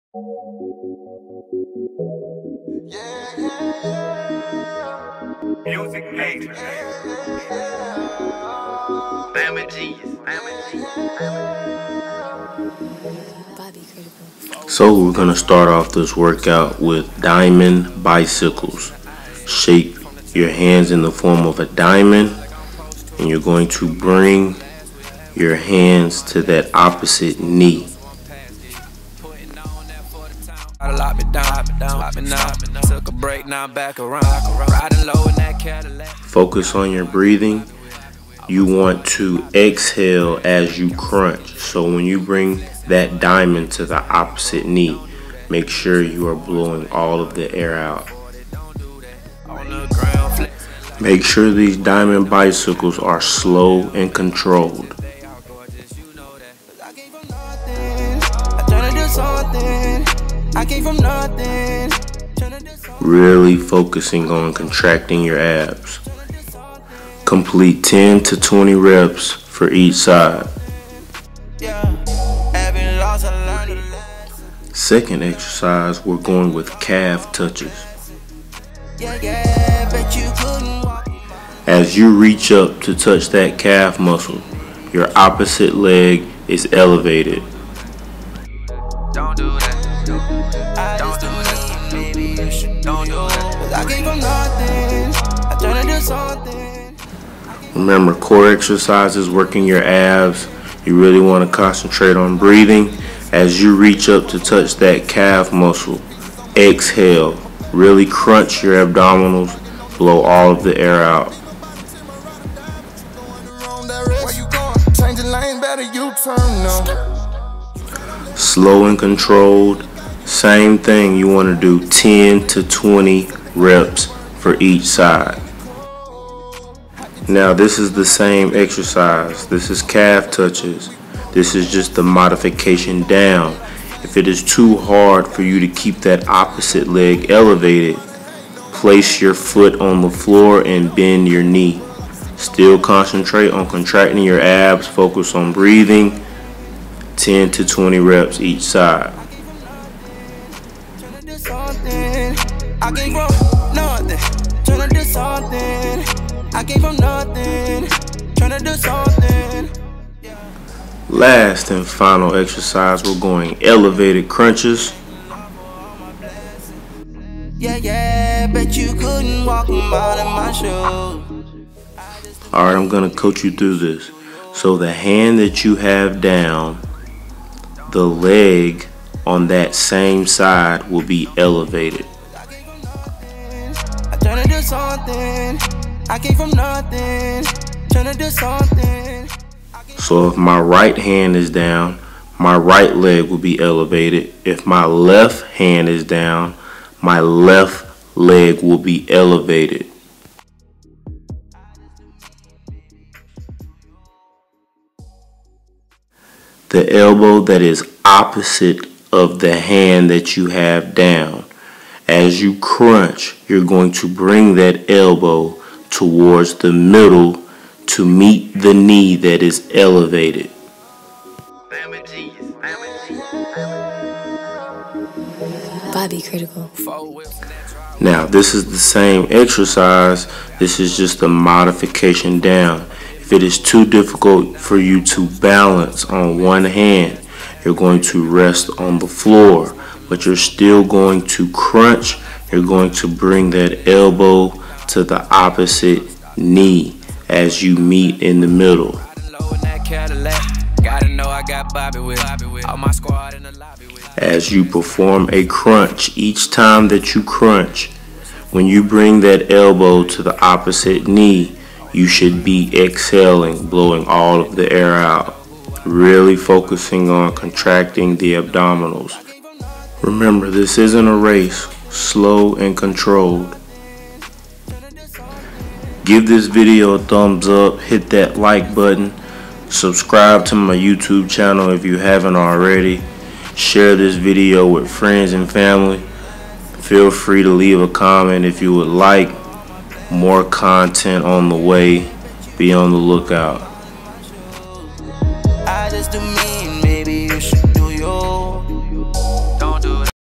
CREATIVE AB WORKOUT - HOME OR GYM
1. Diamond Bicycles
2. Calf Touches
3. Elevated Crunches